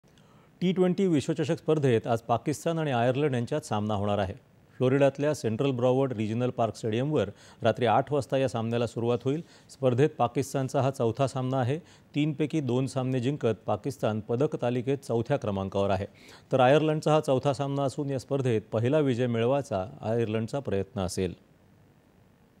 टी २० विश्वचषक स्पर्धा - पाकिस्तान आणि आयर्लंड यांच्यात सामना होणार.
दि.१६.०६.२०२४ :
     टी २० विश्वचषक स्पर्धेत आज पाकिस्तान आणि आयर्लंड यांच्यात सामना होणार आहे. फ्लोरिडातल्या सेंट्रल ब्रॉवर्ड रिजनल पार्क स्टेडियमवर रात्री आठ वाजता या सामन्याला सुरुवात होईल. स्पर्धेत पाकिस्तानचा हा चौथा सामना आहे. तीनपैकी २ सामने जिंकत पाकिस्तान पदकतालिकेत चौथ्या क्रमांकावर आहे. तर आयर्लंडचा हा चौथा सामना असून या स्पर्धेत पहिला विजय मिळवायचा आयर्लंडचा प्रयत्न असेल.

8.30am | 11.00am | 1.00pm | 4.30pm | 7.00pm | 9.30pm